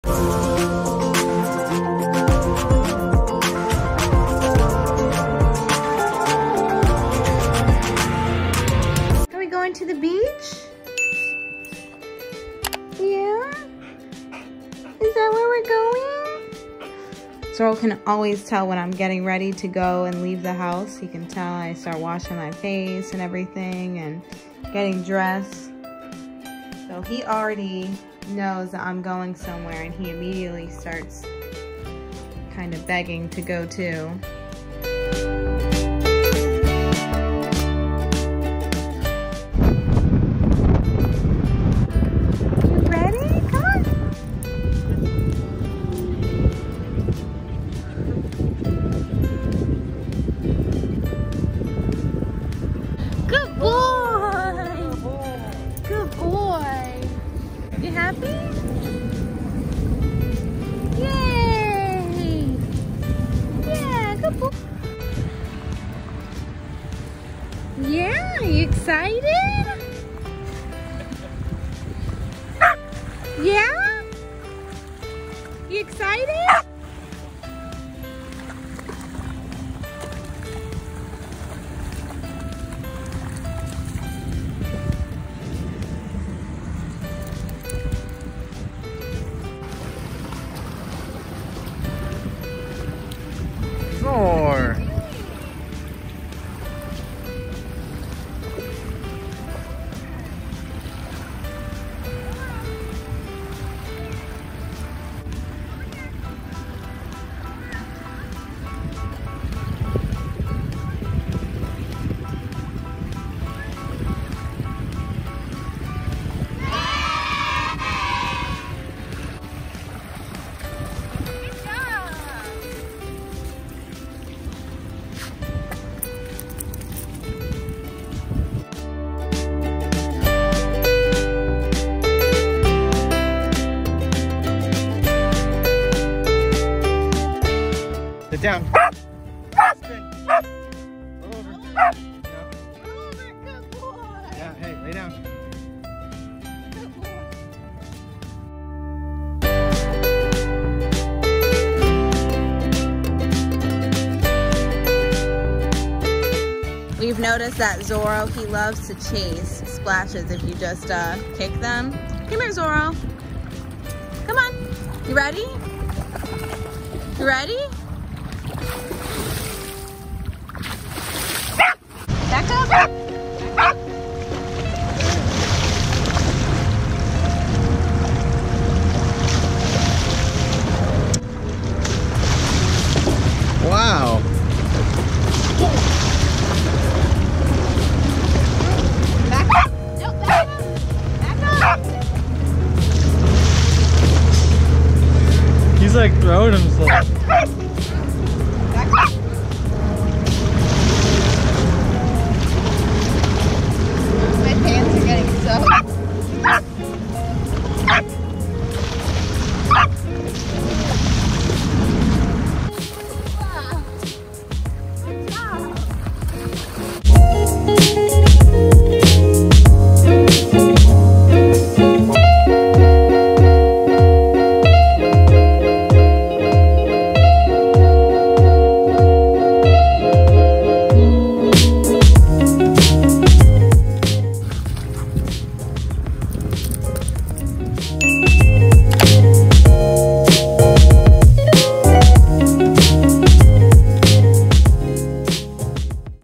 My Blue Heeler's Day on the Causeway! | Cattle Dog Vlog
Today I spent the whole day on the causeway with my australian cattle dog, Zorro! Bringing my dog to the beach is one of my favorite things to do! Zorro kept me smiling all day! Playing with his toy, Biting at the water, and being his joyful quirky self! Doggo days are the best days!

Catch Zorro on Instagram: @zorrotheblueheeler
Find him on Facebook: Zorro the Blue Heeler